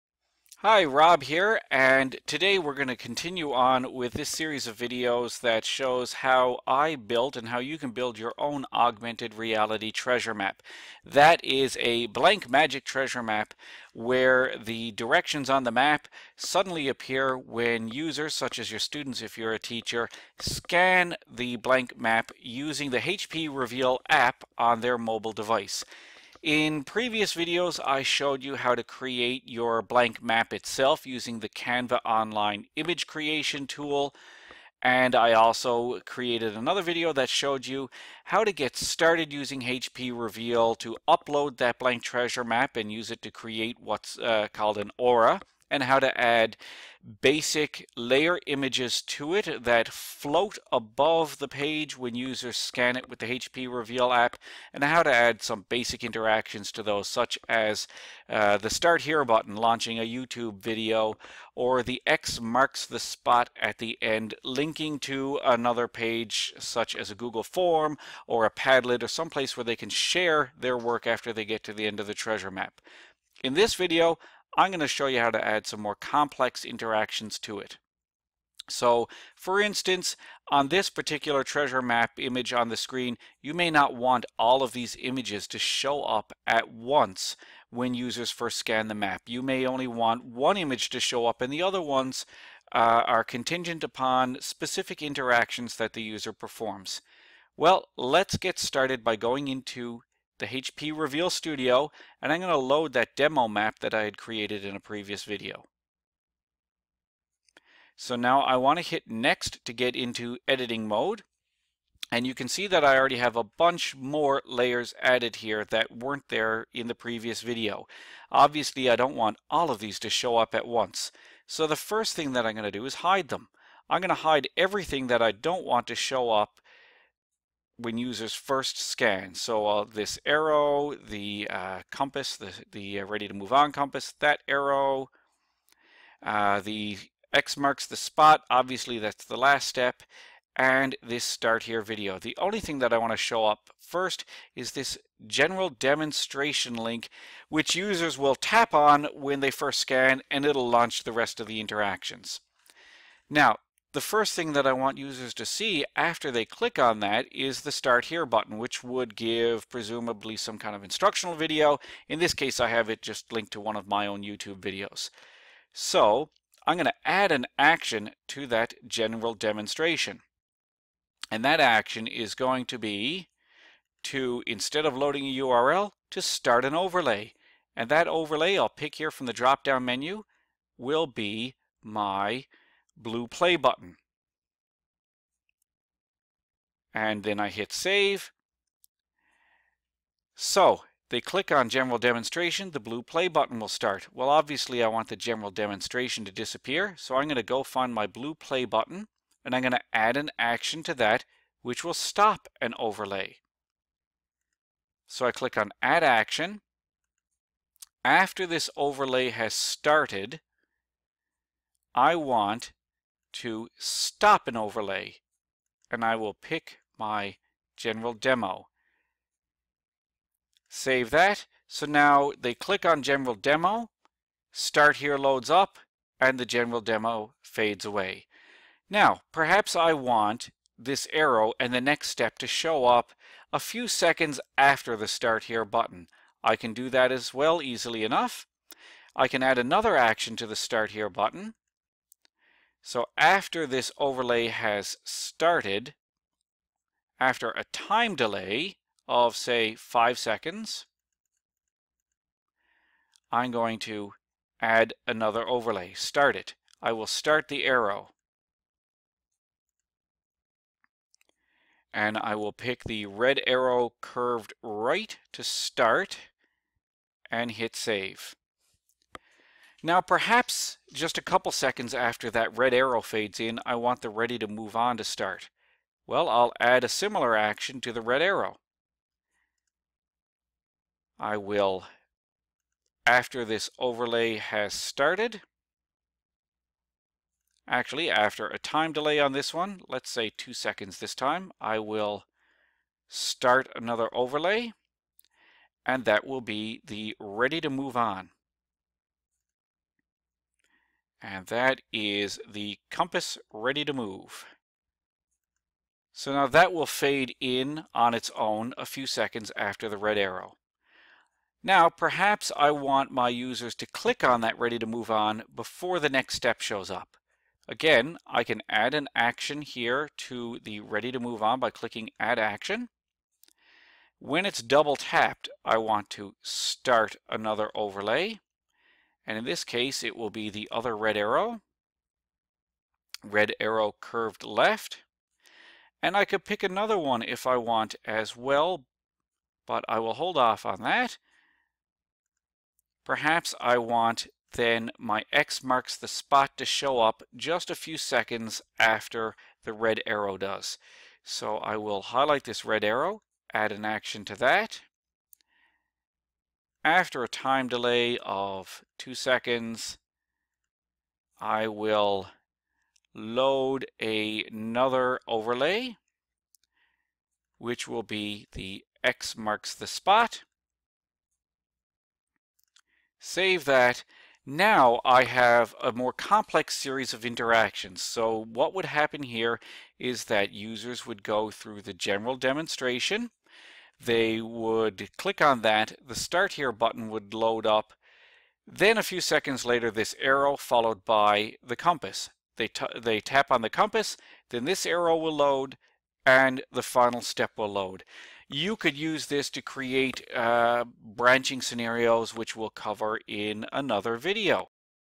Adding Delays and Conditional Triggers to a treasARmap
This demonstration video shows how to add hidden layers and conditional interactions when creating an Augmented Reality treasARmap using the HP Reveal Studio.